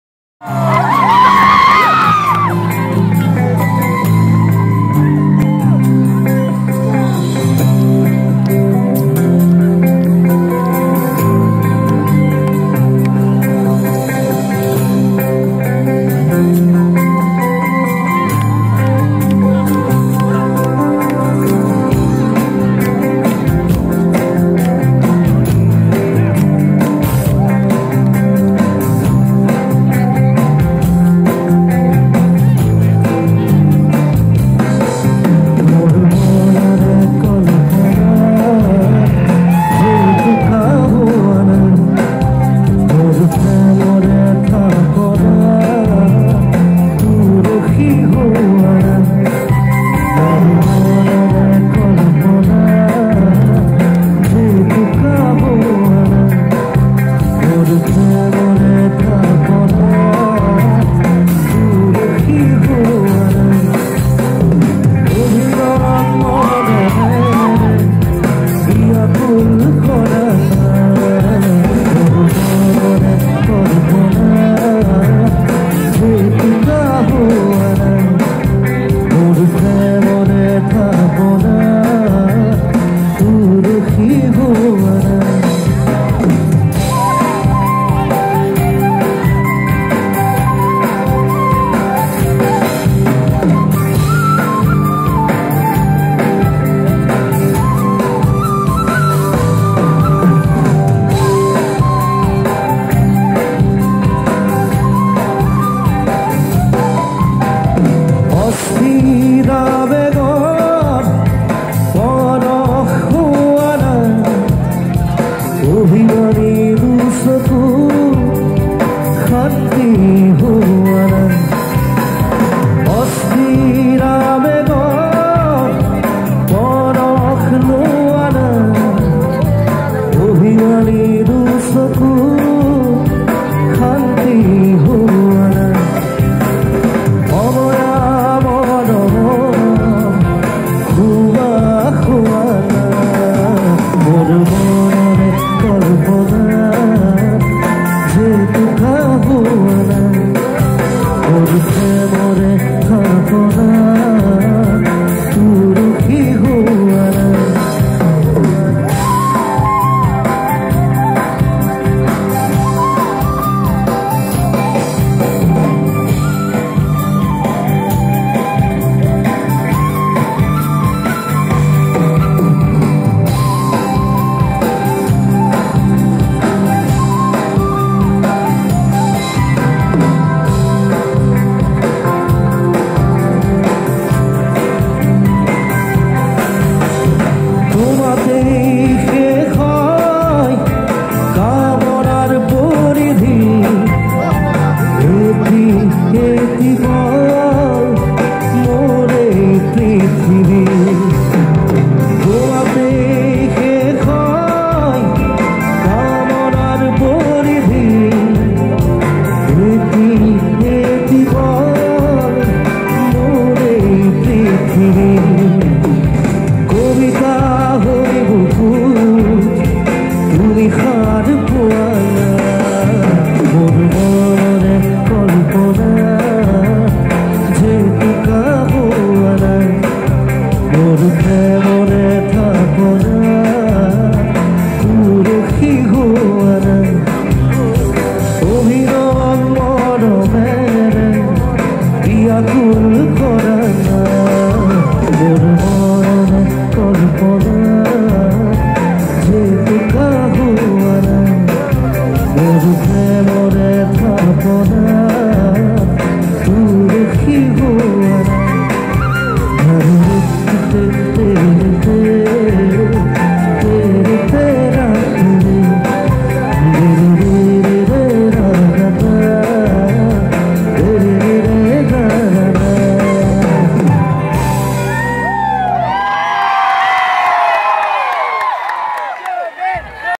ZUBEEN GARG BIHU LIVE 2018
Zubeen Garg (born 18 November 1972) is an Assamese singer, composer, songwriter, music producer, film director, film producer and actor from Jorhat, Assam. His contributions are mostly attributed in Assamese, Bengali and Bollywood. Besides, his songs in Assamese,Zubeen Garg has sung many songs in other languages . This bihu function held at Ganeshguri in Guwahati,Assam